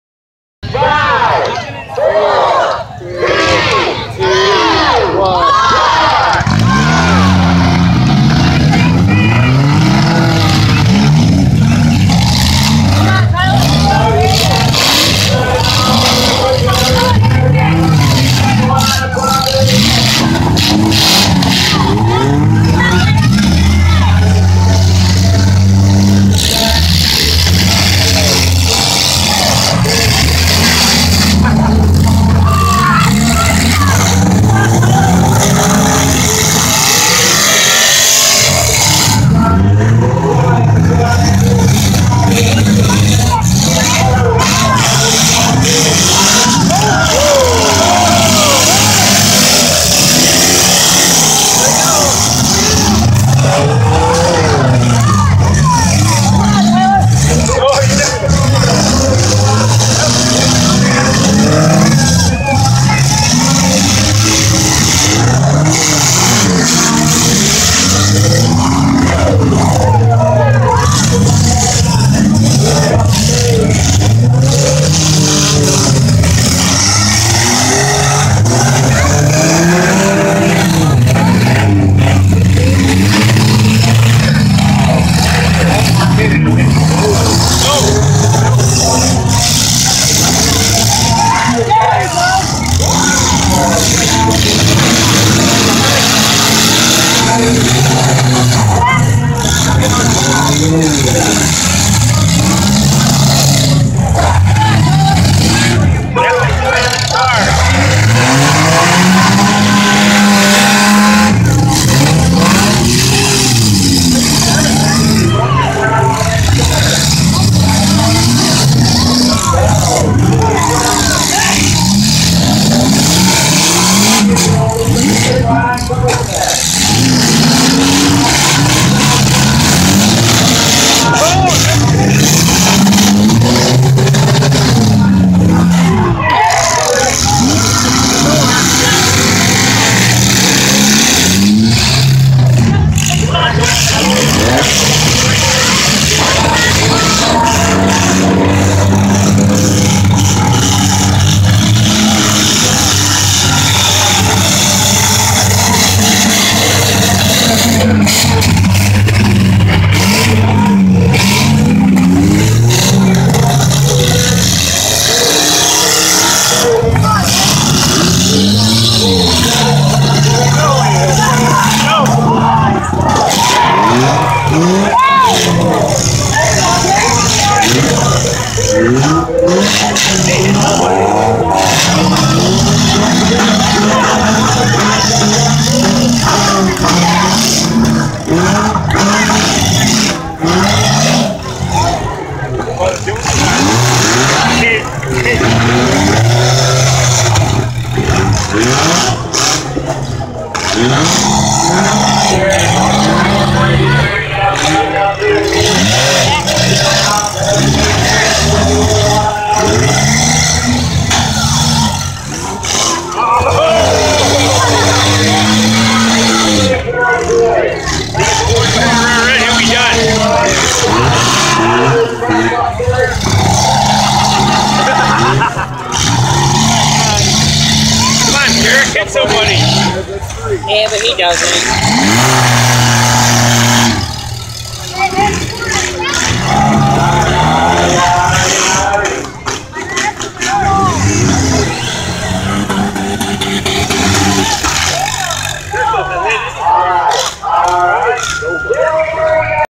Yates county demo 3rd heat 4&6
Yates county fair 2016 demolition derby with 4 and 6 cylinders